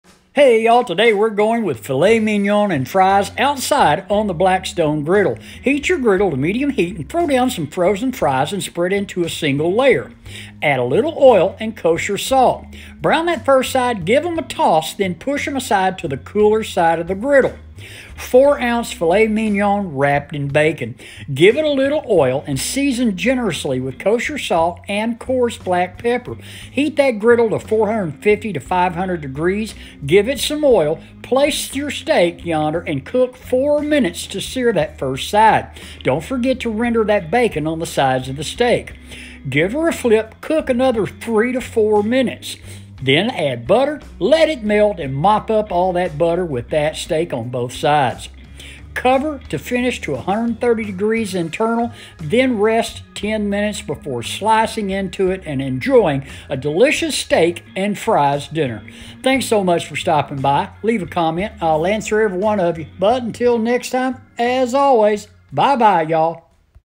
FILET MIGNON & FRIES - Blackstone Griddle Recipe
It's spring time and there's nothing better than heading outside and cooking up some delicious filet mignon and fries on the Blackstone griddle.

Shopping List:
4 oz. filet mignons at least 1 1/2- 2 inches thick (choice or prime cut)
Strip of thick cut bacon (1 per steak)
High temp cooking oil
Kosher salt and course black pepper
Salted butter
Frozen French fried potatoes

Directions:
Wrap each file with strips of bacon (secure with either tucking in the end or with a toothpick)
Generously season the filets with salt and pepper.
Let set out 45 minutes to one hour to come to room temperature.
Preheat griddle on medium.
Add a generous amount of oil to the pre-heated griddle and add the frozen fries, arranging them on a single layer. Toss after they have browned. Season with salt. When crispy, push to the side of the griddle to keep warm.
Increase the heat to high and allow to heat up. You're wanting 450-500 degrees surface temperature.
Apply a little oil to the griddle surface and place the room temperature filets. Gently press them down to make good contact.
Depending on the thickness of the filets, sear 3-4 minutes per side. Tip: when they release easily, it's time to flip.
Once seared, move the filets to the cooler side of the griddle, add a pat of butter to the top, cover and cook to your desired temperature. My target is 135 for medium rare, but I will remove at 130 to rest. They will continue to rise about 5 degrees while resting.

Standard License Background Eraser software: available through Apple App, Seller:  Tasnim AhmedFilmora.com/io  (Wonderland)
Thumbnail Image: Phonto, available from Apple Store